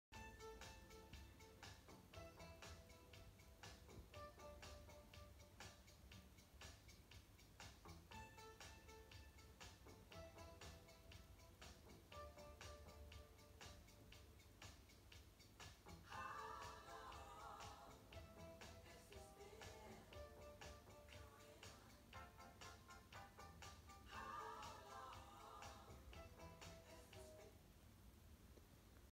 LG OLED 55B7 Banding Test2
What do you think about this amount of banding on my B7 is it acceptable?